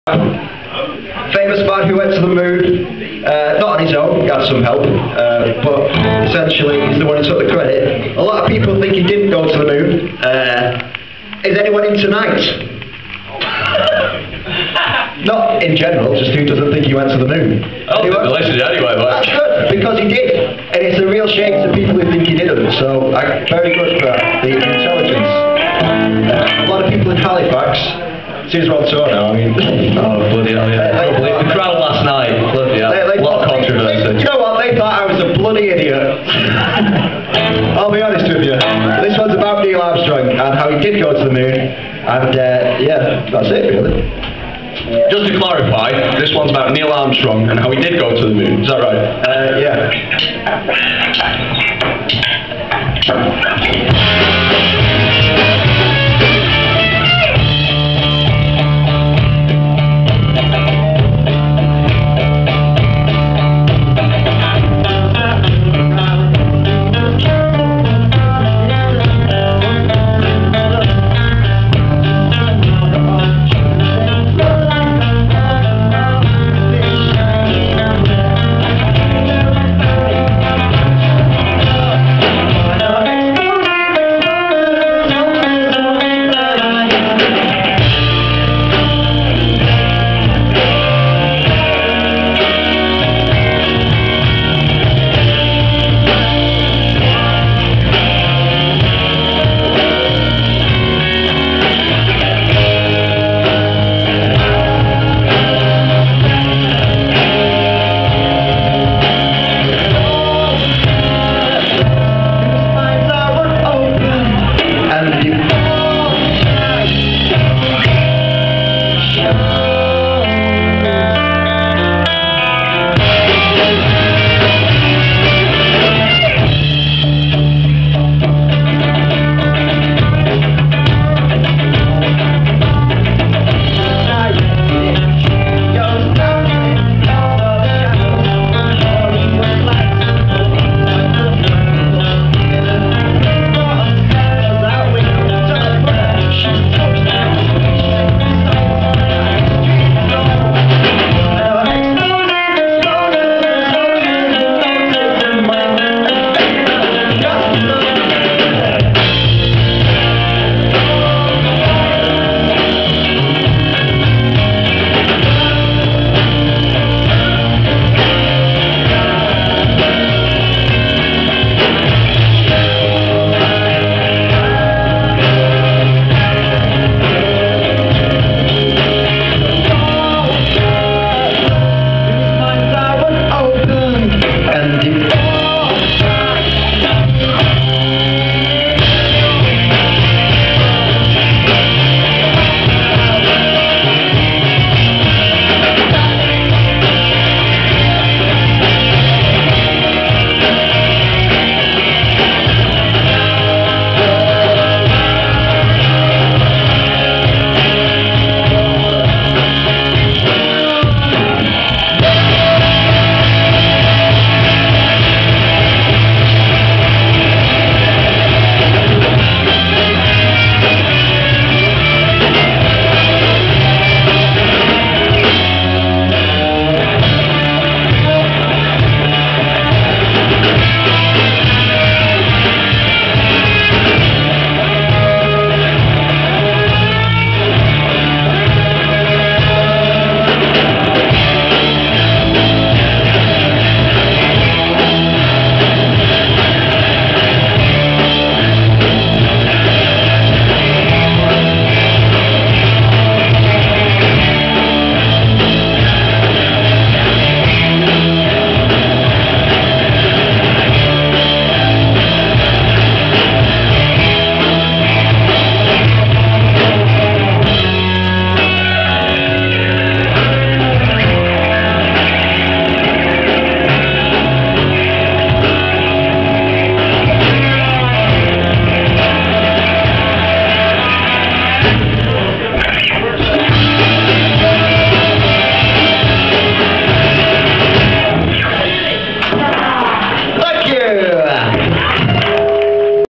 The Planets
The Planets live at Bury village hall, Ramsey, Cambs. Sunday 29th August 2010, "There goes the Summer" gig.